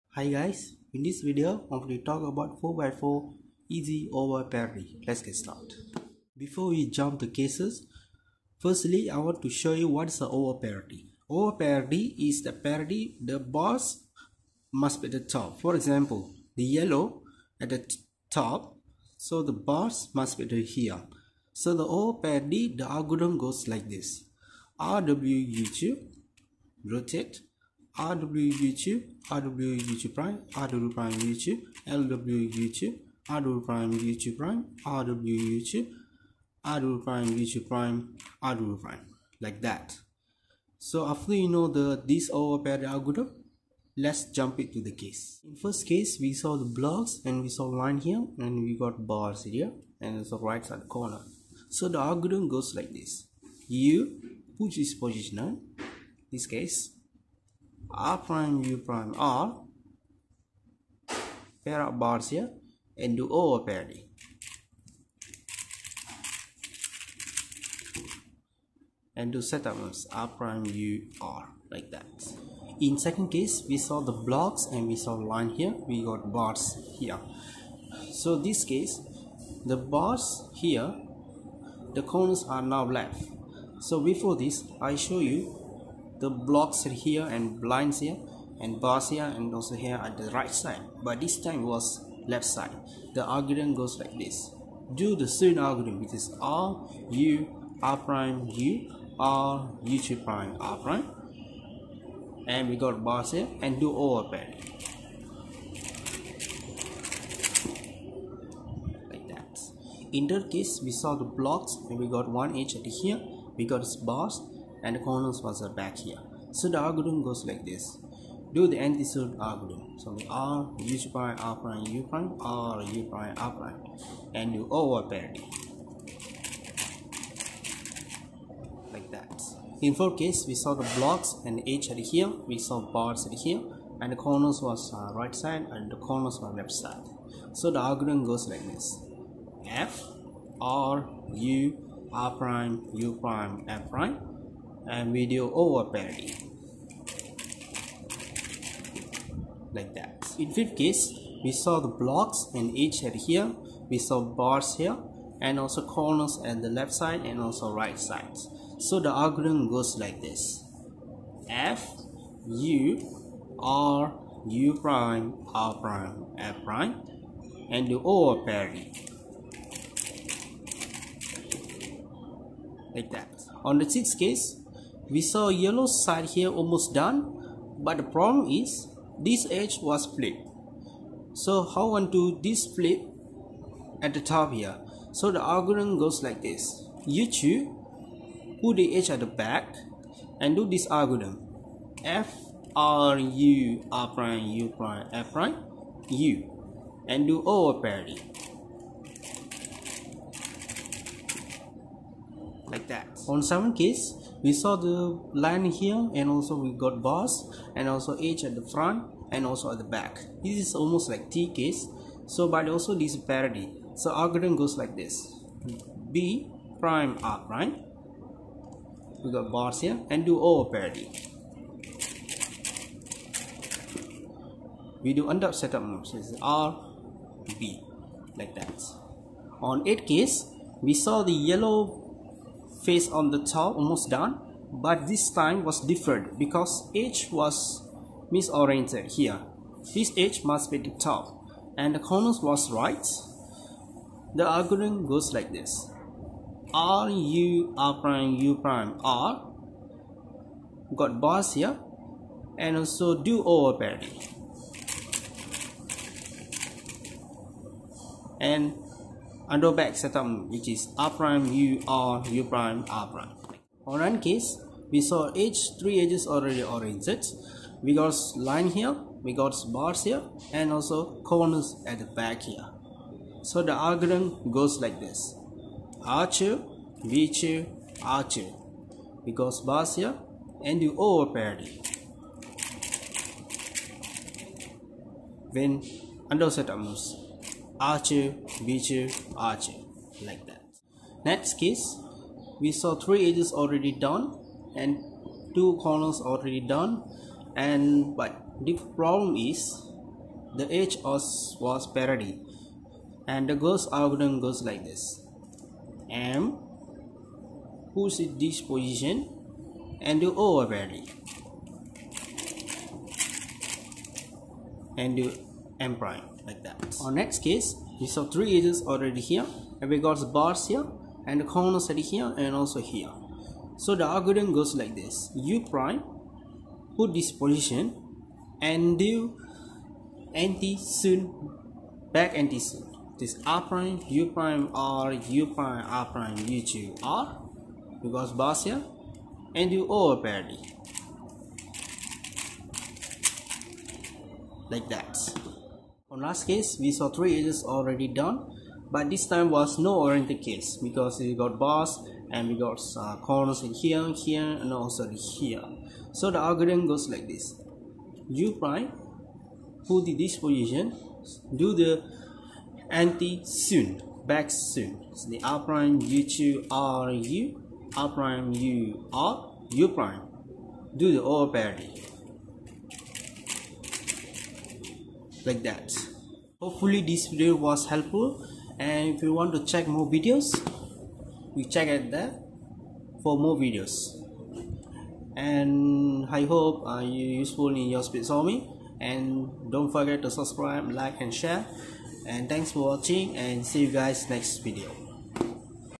12 Useful OLL Parity Case Make YOU BETTER LOOKAHEAD
Time Stamps

0:00 - Intro
0:06 - OLL Parity Algorithm
0:45 - Case 1
1:12 - Case 2
1:50 - Case 3
2:10 - Case 4
2:30 - Case 5
2:54 - Case 6
3:24 - Case 7
3:49 - Case 8
4:24 - Case 9
4:51 - Case 10
5:17 - Case 11
5:46 - Case 12
6:16 - Outro